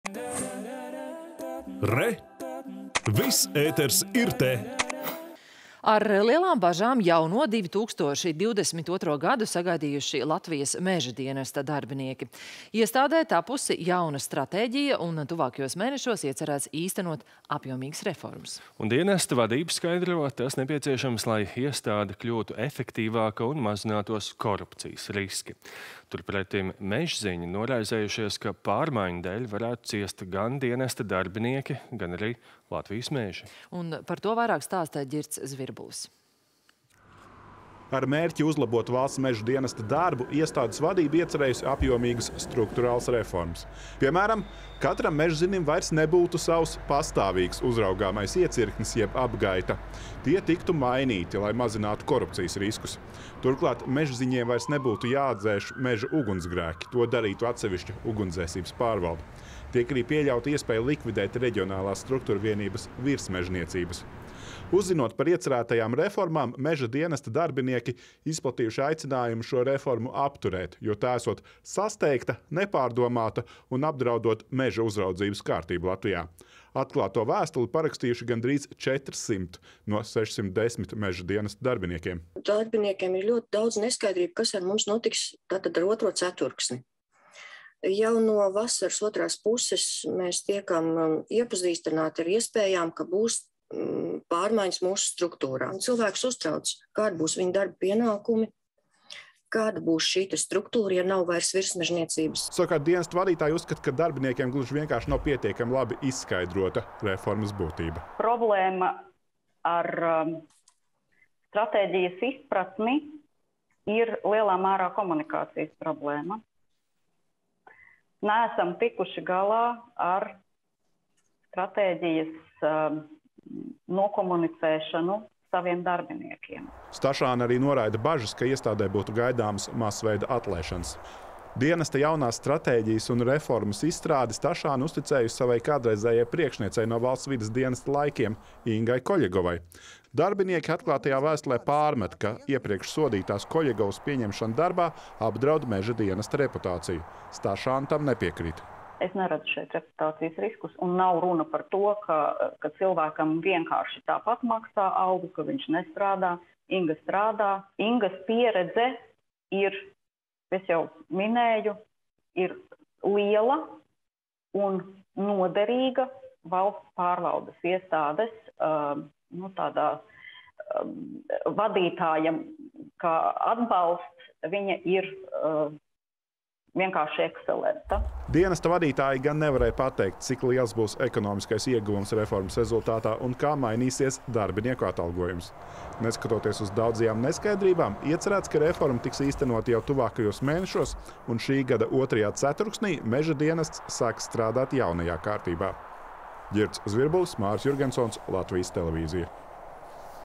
Meža dienests – reformas priekšā
Ar lielām bažām jauno – 2022. gadu sagaidījuši Latvijas meža dienesta darbinieki. Iestādē izstrādāta jauna stratēģija un tuvākajos mēnešos iecerēts īstenot apjomīgas reformas. Dienesta vadība skaidro – tas nepieciešams, lai iestāde kļūtu efektīvāka un mazinātos korupcijas riski. Turpretim mežziņi noraizējušies, ka pārmaiņu dēļ varētu ciest gan dienesta darbinieki, gan arī – Latvijas meži. Turpina Ģirts Zvirbulis.